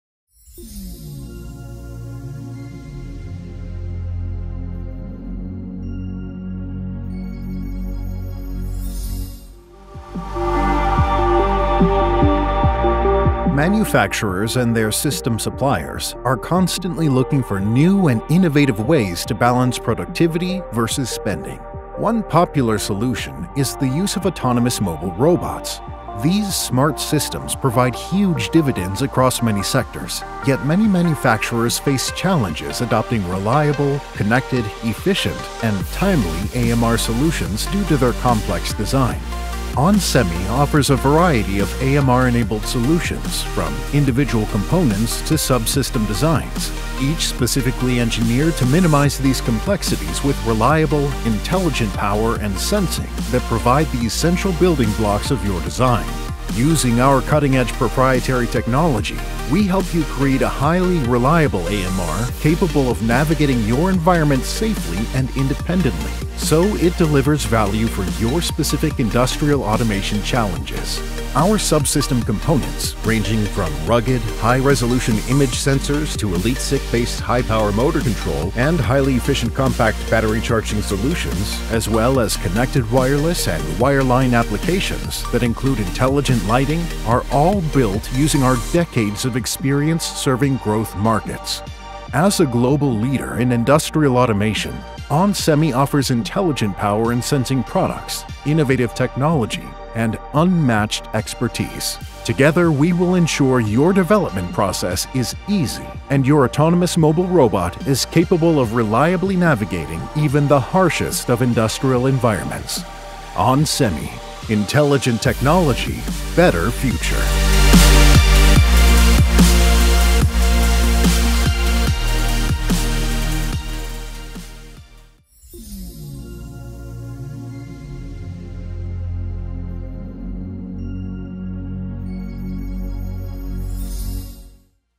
onsemi's Autonomous Mobile Robots (AMR)
Manufacturers and their System Suppliers are constantly looking for new and innovative ways to balance productivity vs. spending. One popular solution is the use of Autonomous Mobile Robots (AMR). These smart systems provide huge dividends across many sectors.